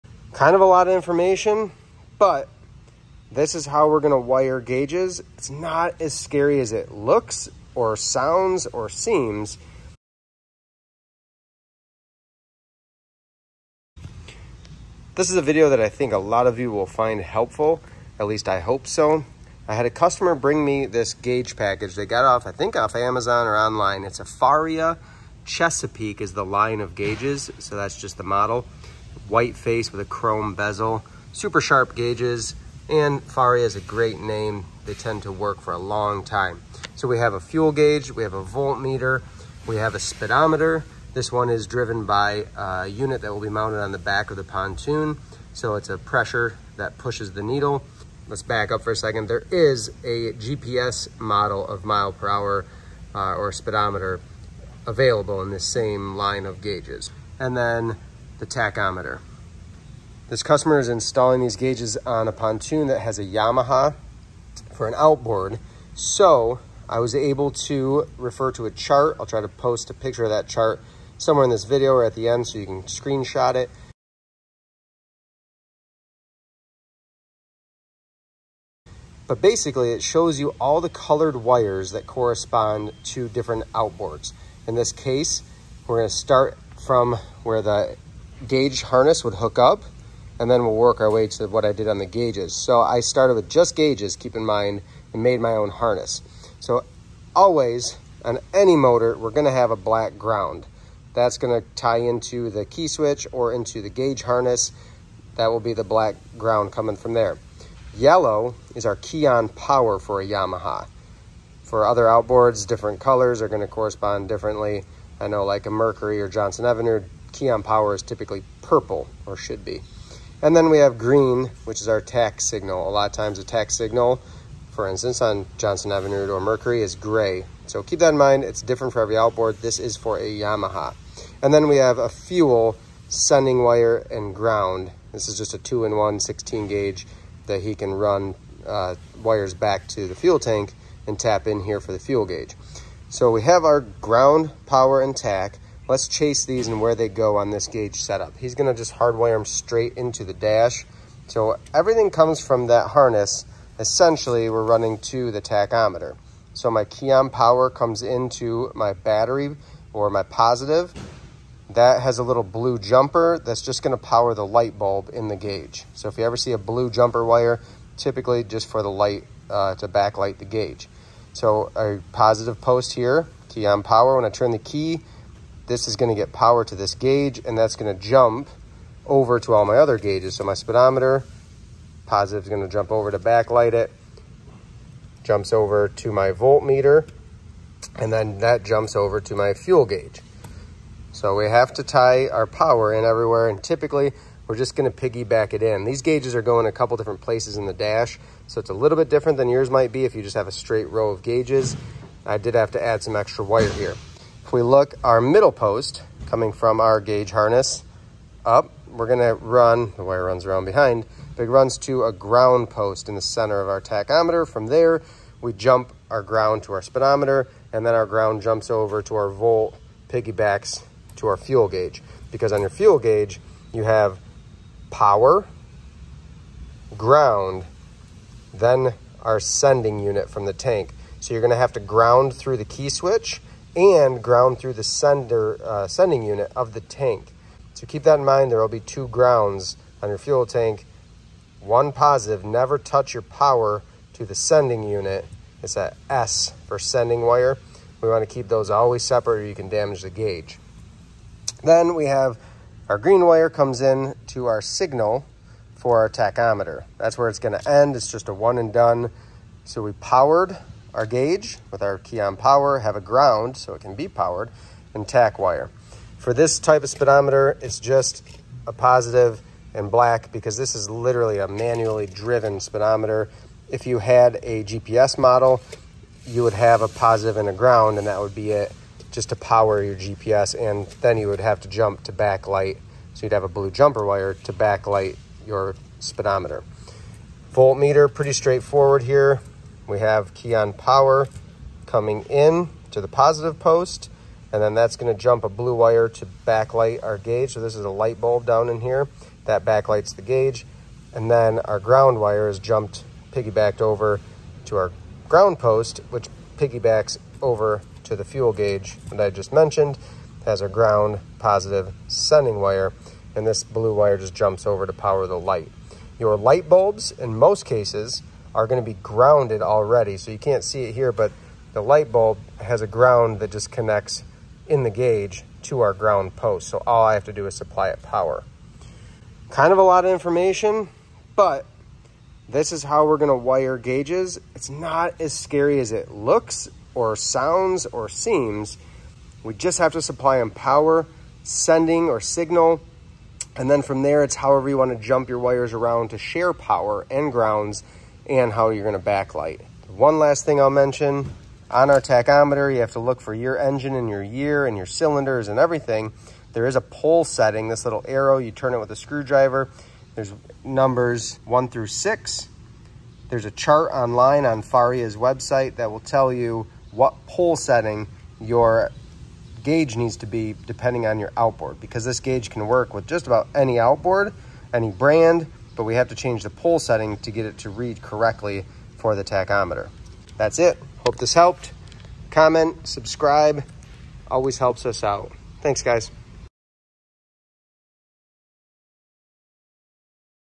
WIRING BOAT GAUGES - Yamaha Outboard Faria Gauge Package
DON'T BE AFRAID TO WIRE YOUR OWN GAUGES! We're breaking down the basics of what's happening behind the dash on your console with your gauge wiring. It's not so scary once you understand how gauges are powered and supplied signal. These gauges are Faria brand, and their Chesapeake line. I've had great luck with installing them on lots and lots of boats (even my own!).

Say Thanks by Contributing Financially to Tom’s Toons Channel